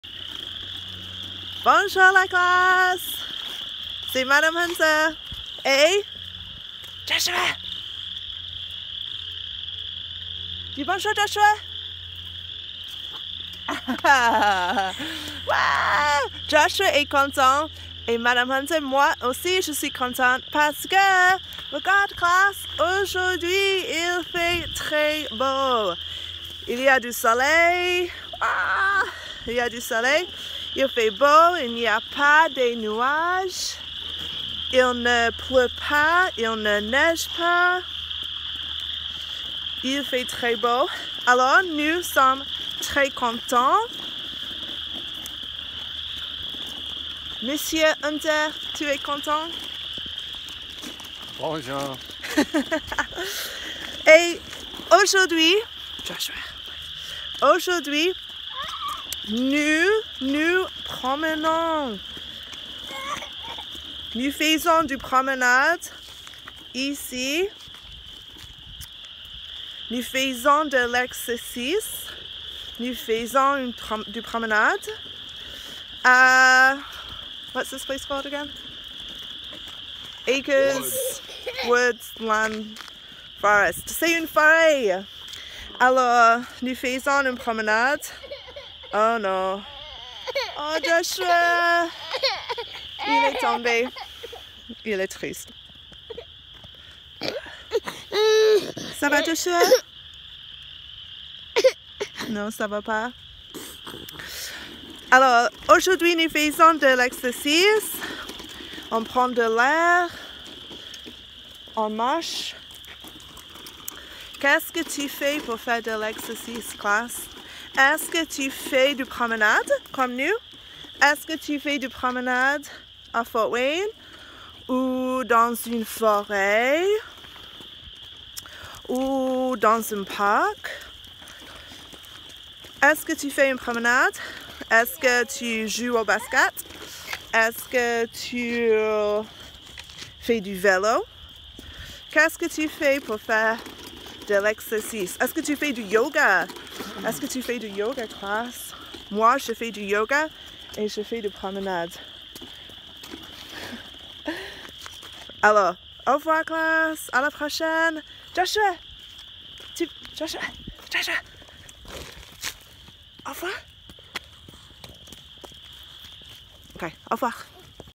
jeudi 26 mars - est-ce que tu fais de l’exercice?
Mme Hunter et sa famille se promènent dans une forêt à Fort Wayne. Qu’est-ce que tu fais pour l’exercice?